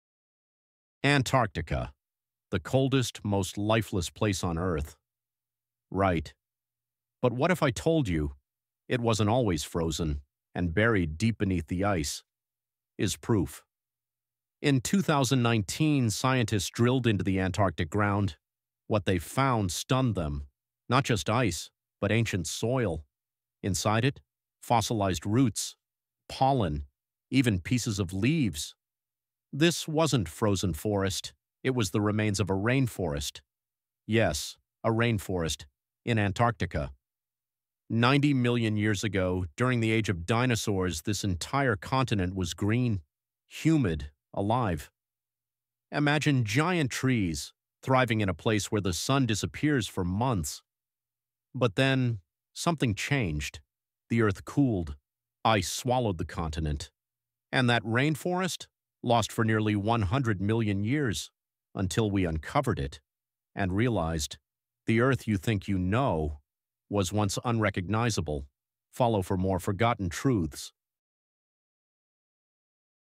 THERE'S A Rainforest Underneath Antarctica… and It Changes Everything
Beneath Antarctica’s frozen surface… scientists found something they never expected.

Fossilized roots. Ancient pollen. Pieces of leaves — preserved not in ice… but in soil.

90 million years ago, this frozen continent was a lush, living rainforest — filled with trees, life, and darkness.

What happened next changed Earth forever.

🎙️ Follow Fact Nomad for more forgotten truths, ancient discoveries, and mind-blowing science.

Science,
Antarctica,
forest,
rainforest,
fact,